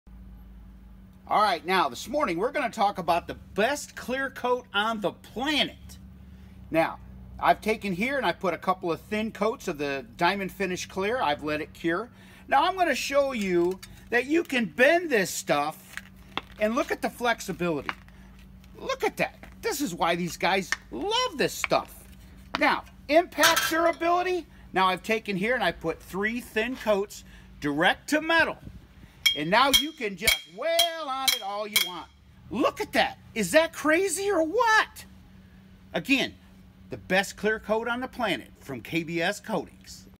DiamondFinish Clear - Showing the Insane Durability and Flexibility of this Clear Coat
This video shows the durability and flexibility of DiamondFinish Clear. DiamondFinish Clear is a tough Clear Coat that can be applied Direct To Metal or over other coatings. DiamondFinish Clear is a water clear high gloss clear coat that remains permanently flexible and is able to perform in the toughest environments without yellowing, cracking or peeling.